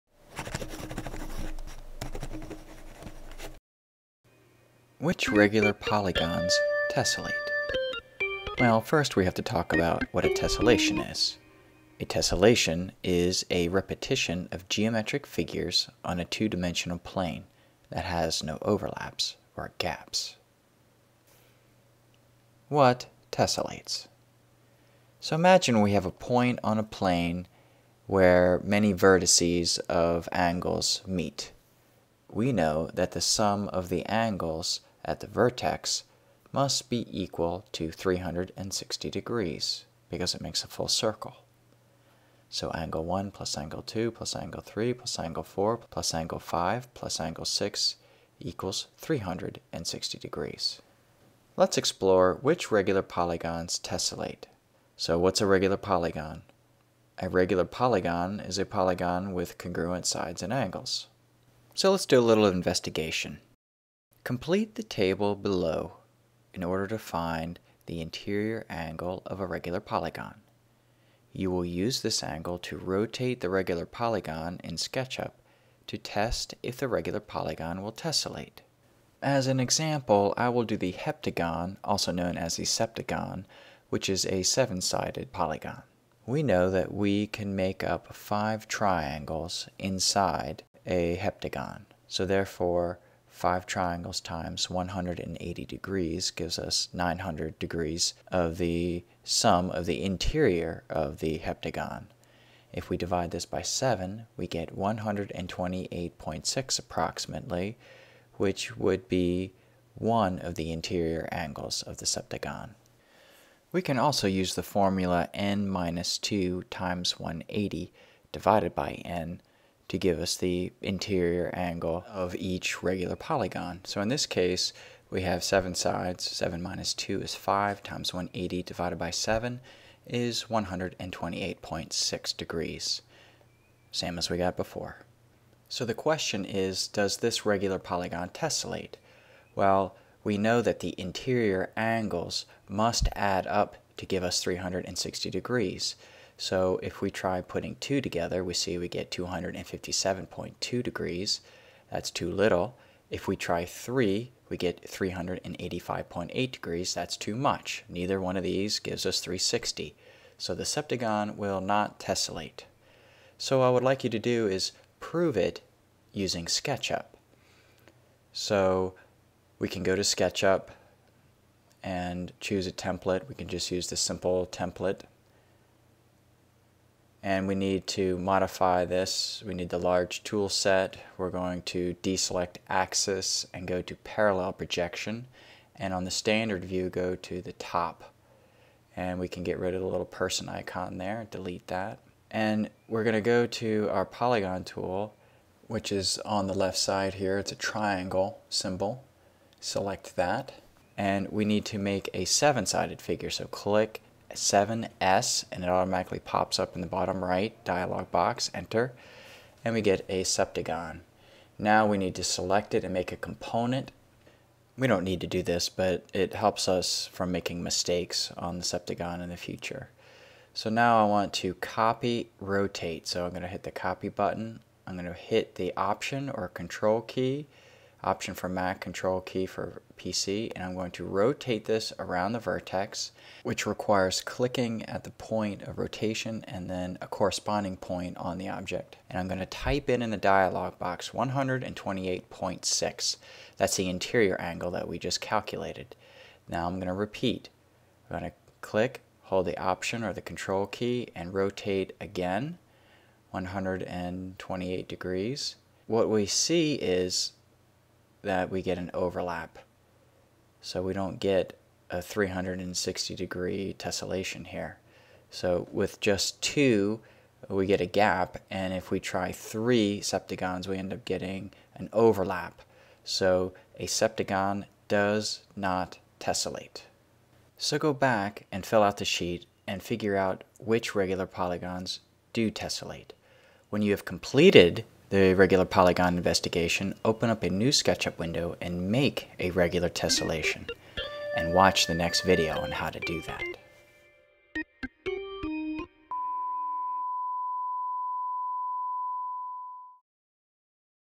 10.6 Video Investigation "Which Regular Polygons Tessellate?"
This is an investigation on which regular polygons tessellate.  SketchUp is used to prove it.